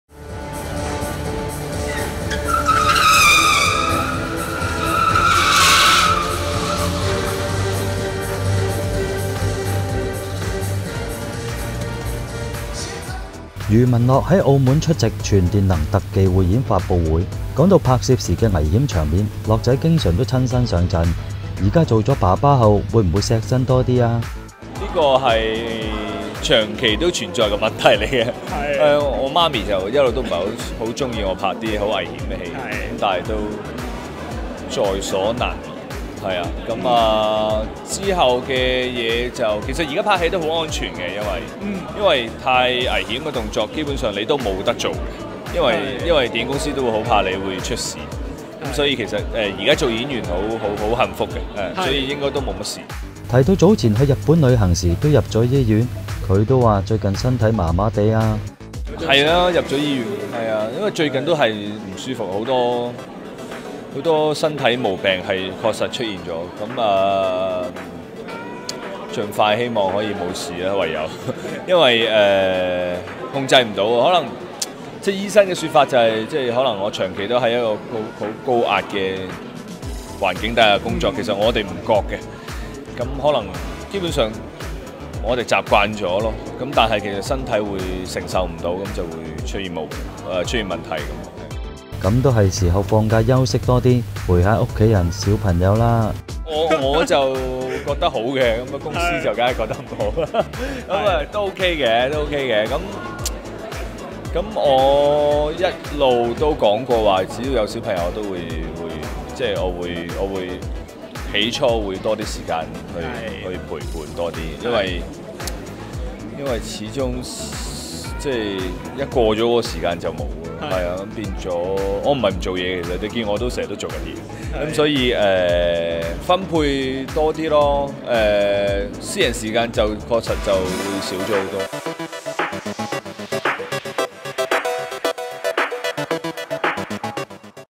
【娛樂】身體欠佳 余文樂 : 多啲時間陪小朋友 2018-10-19
余文樂出席「全電能特技匯演發布會」，講到拍戲時要拍攝危險場面，樂仔經常親身上陣。不過樂仔話，現時拍戲都好安全，因為電影公司都驚演員會出意外。

問到他早前在日本入院，樂仔話 : 「身體好似唔太好，醫生話我可能長期處於高壓力環境下工作，令身體受不了。我依家希望抽多點時間陪小朋友，因為過咗一段時間，小朋友大個咗，就變咗少時間了。」

universalproductionmusic